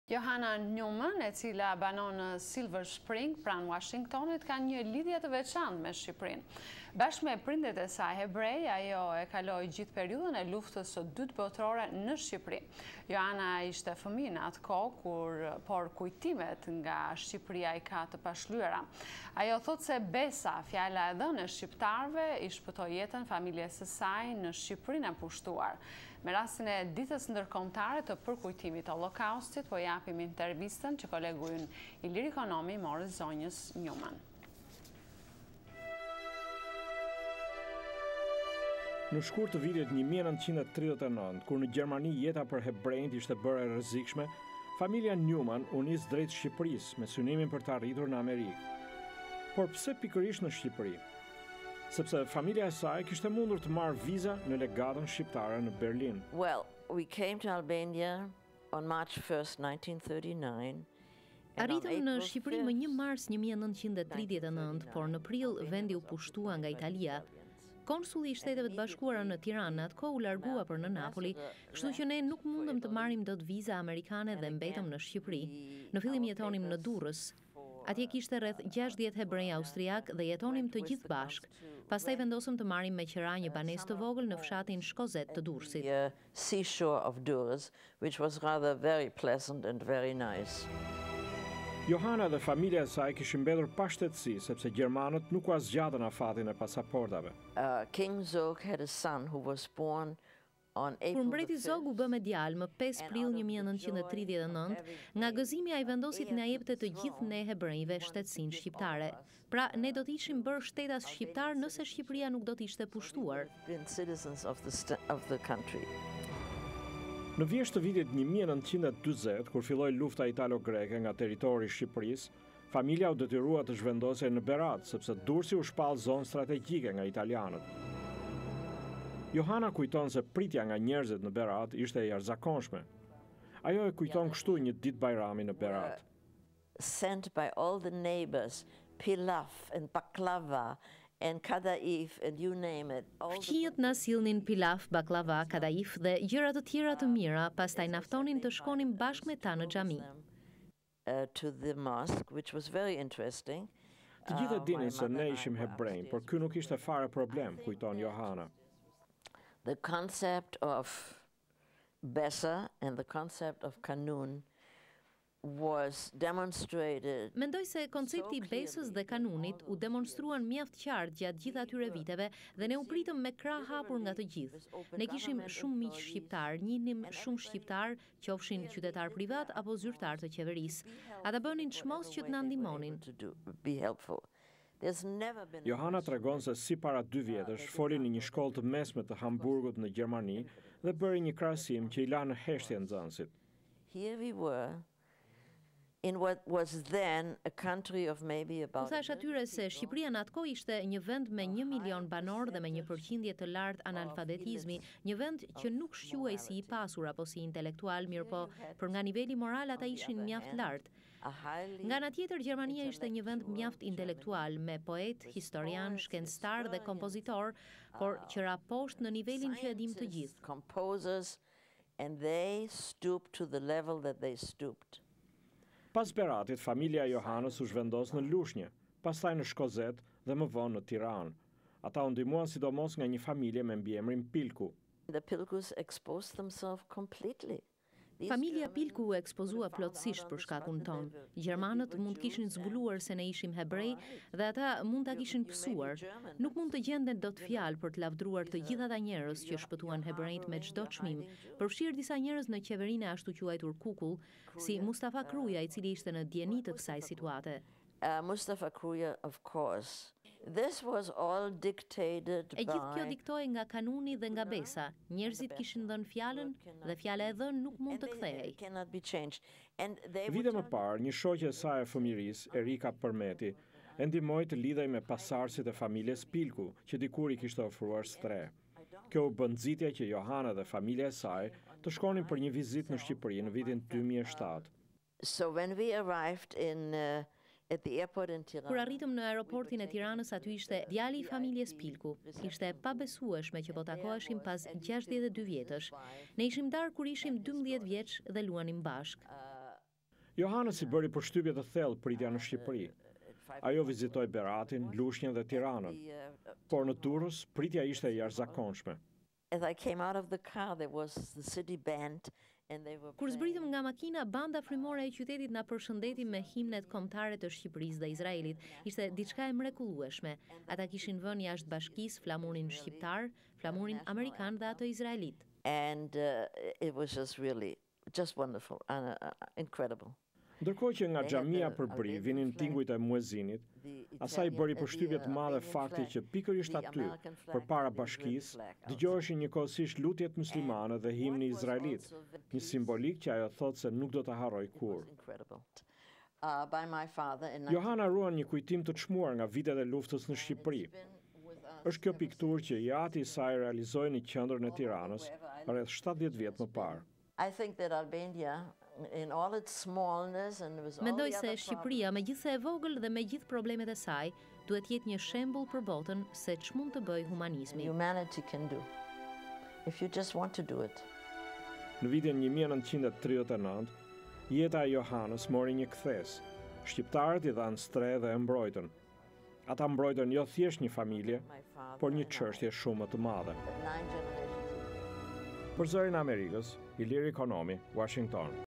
Johanna Neumann - kujtimet nga Shqiperia
Johanna Neumann, e cila banon në Silverspring afër Uashingtonit, ka një lidhje të veçantë me Shqipërinë. Bashkë me prindërit e saj hebrej, ajo e kaloi gjithë periudhën e Luftës së Dytë Botërore në Shqipëri.